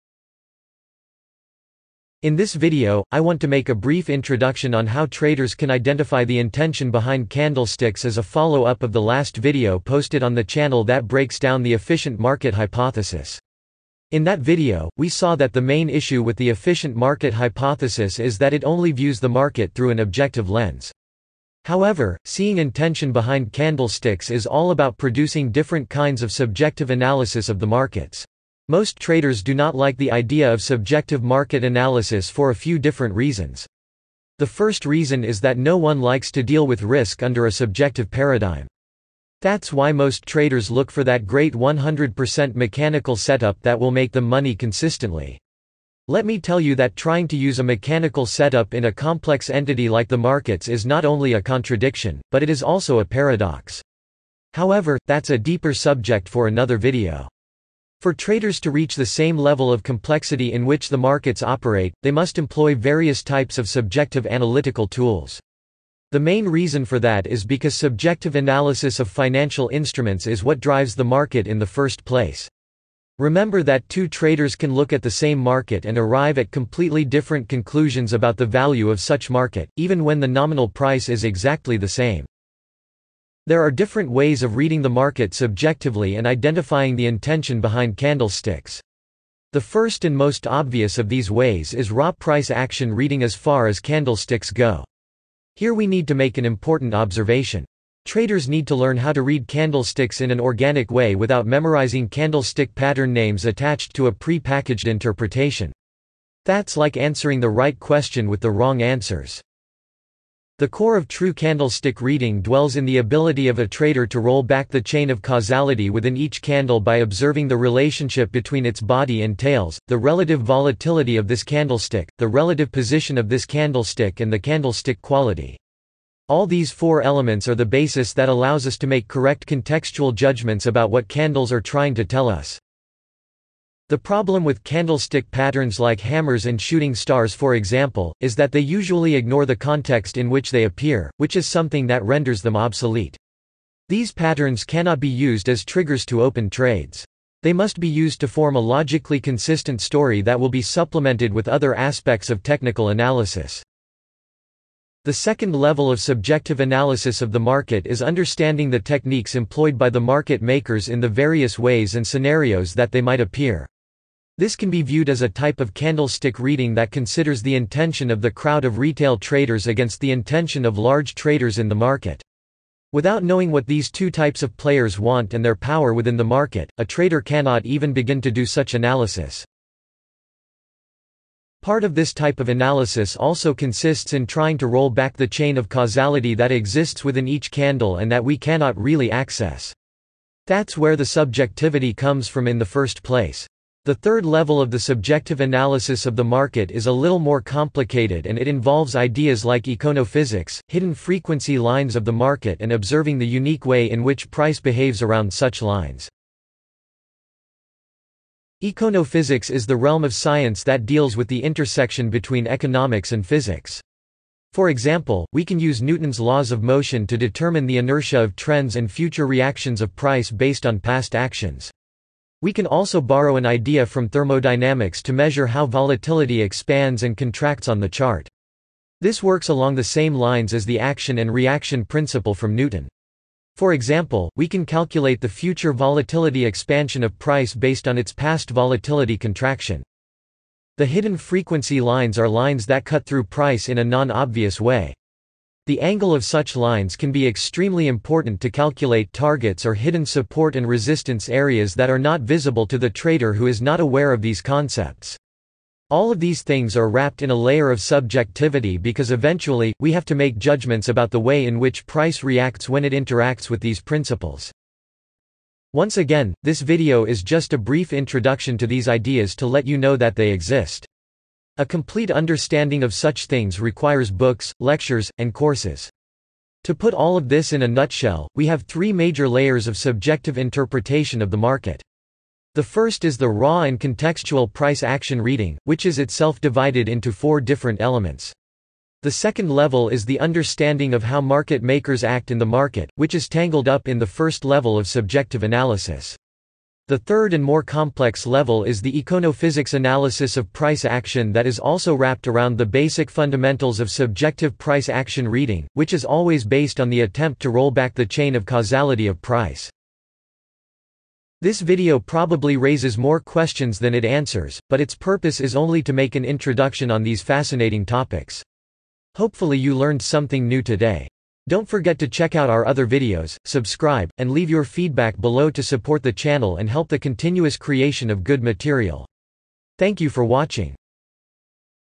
3 Main Levels of Subjective Analysis of the Market
This is a video about, forex candlestick patterns, forex market makers and econophysics.

In order to be able to read the intention behind candlesticks, traders must understand how to read candlestick in an organic way, identify forex market makers and their deceptive techniques, and learn about econophysics.